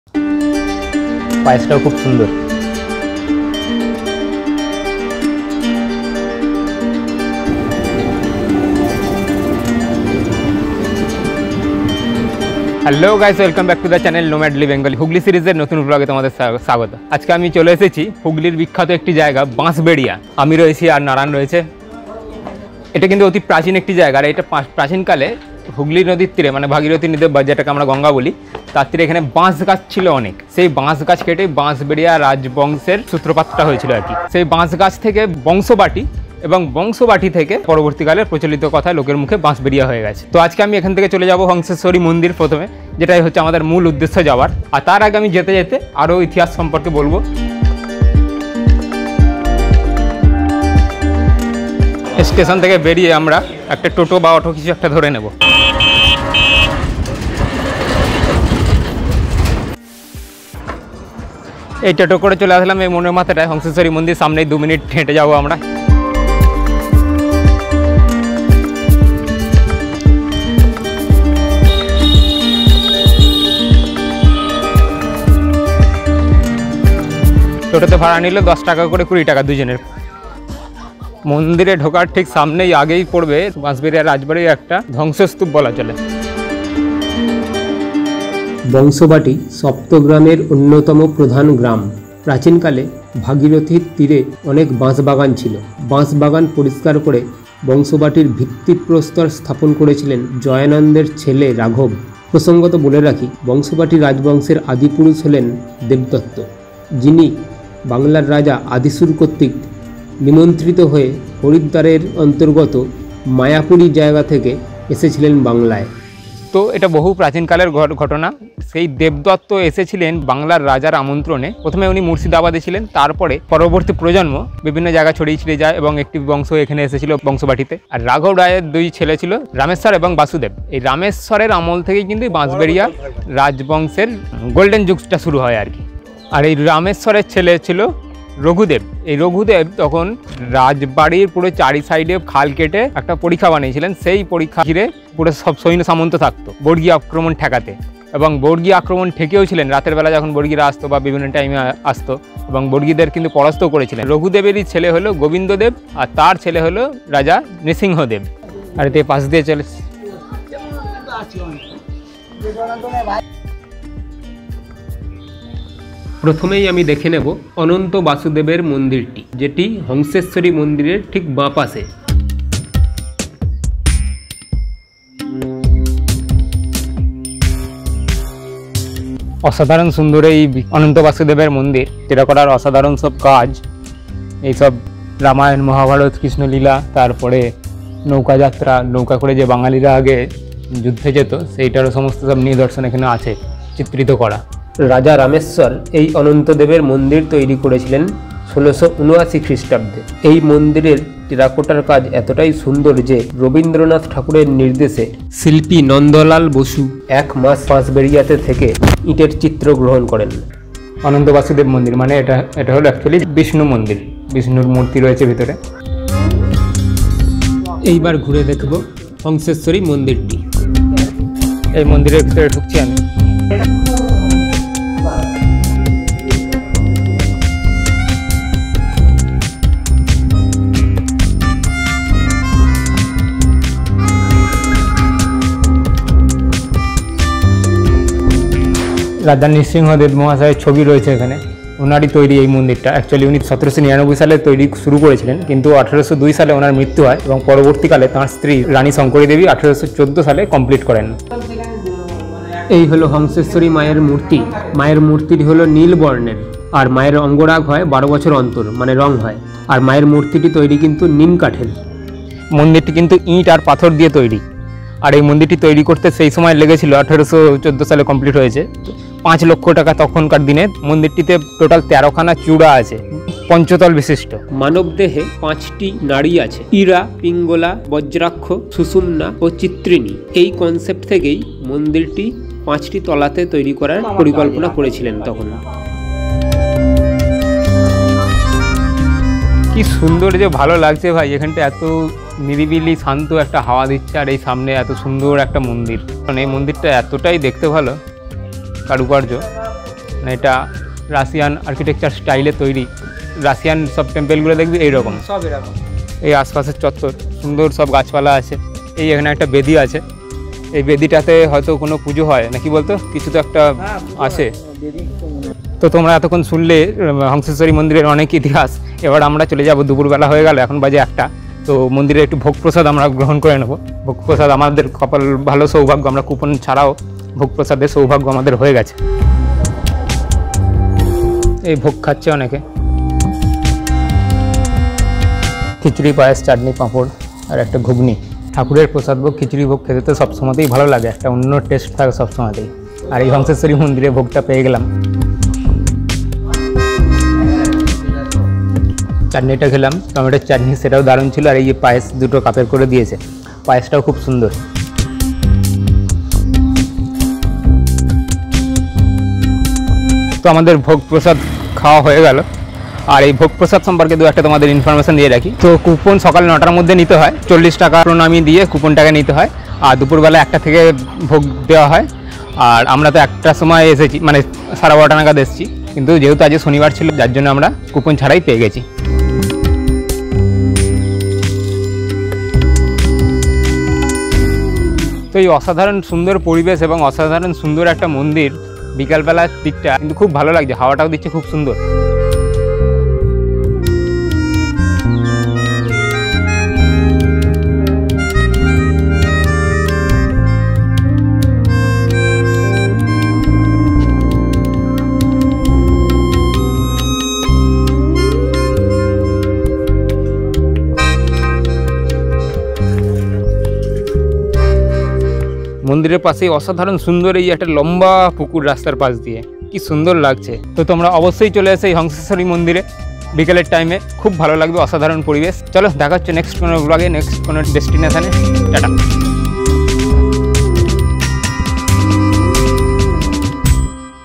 Hangseshwari temple in Bansberia Hooghly | Russian architecture style | হংসেশ্বরী মন্দির
Hangseshwari temple, Ananta basudev mandir in Bansberia, Hooghly.

Watch Nomadli Bengali travel series in below link –
Nomadli Bengali চ্যানেলের অন্যান্য ট্রাভেল সিরিজগুলি দেখার জন্য নিচের লিংকগুলিতে ক্লিক করো -